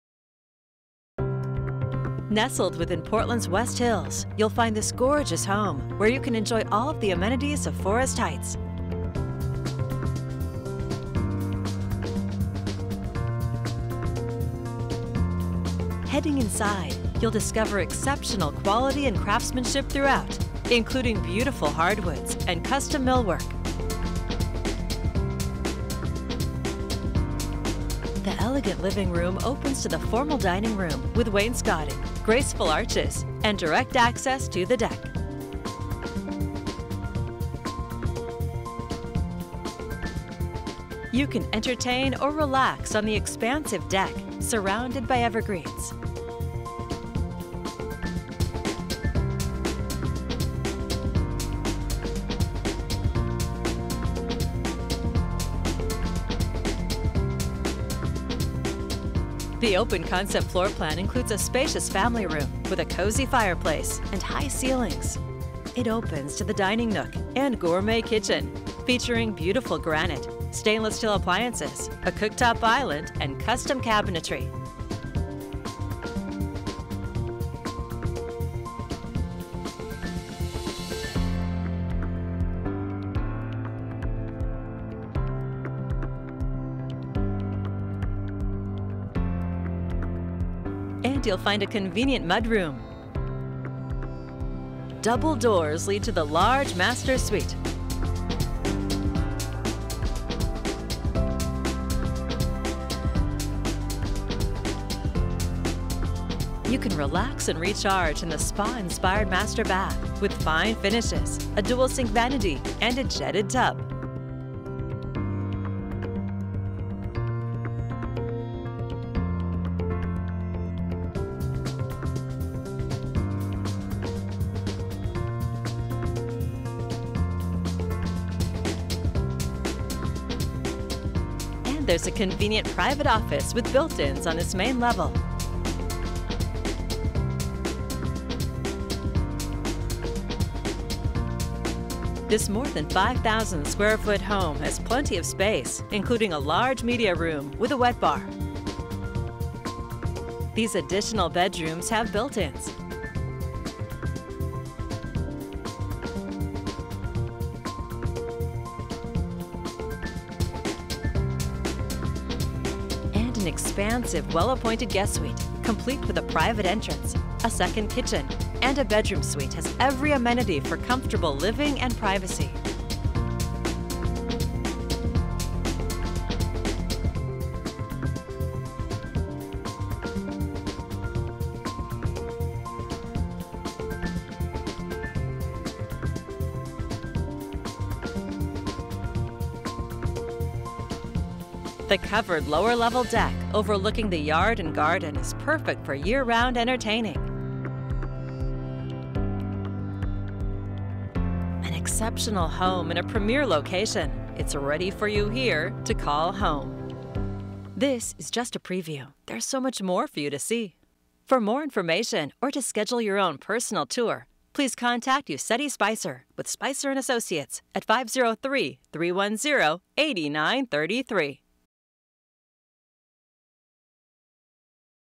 Stunning Home in Portland's West Hills ~ Video of 8534 NW Timber Ridge Ct.
Nestled within Portland's West Hills & surrounded by evergreens, Forest Heights offers a prosperous community that is perfect for raising a family. Stunning home offering quality & craftsmanship throughout including hardwood, granite, custom cabinetry & millwork. Covered porch surround the home. Guest suite w/ private entrance & 2nd kitchen/bedroom & bath. Enjoy all the amenities of Forest Heights community without HOA monthly fees. Contact Yussetty Spicer for your personal tour or more information.